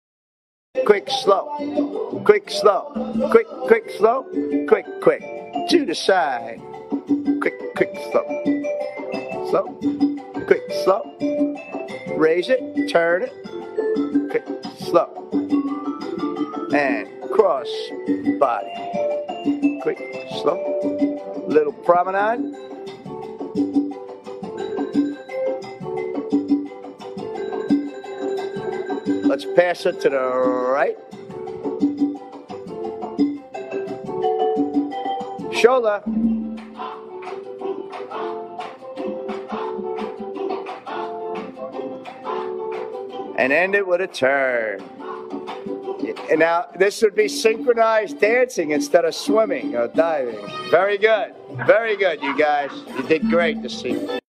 Salsa - Combo Class, July 2012 at Julington Creek Plantation with Instructor Tony Paolucci.
Salsa, Cha Cha and Tango group classes at Julington Creek Plantation Recreation Center in St Johns, Florida with OnStar Production & Entertainment and Tony Paolucci.